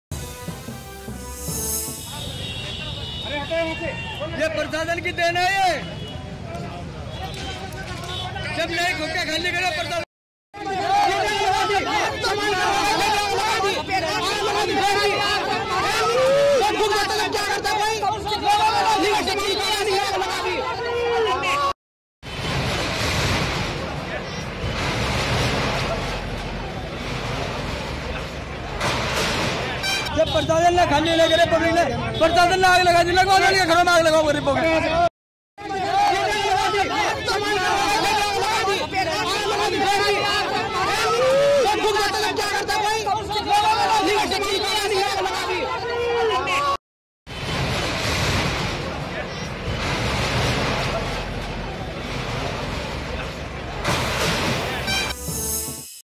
'अतिक्रमण' पर आगजनी - देखिए वीडियो
मुज़फ्फरनगर में अतिक्रमण हटाने पहुंची प्रशासन की टीम और दुकानदारों के बीच तीखी झड़प और आगजनी ।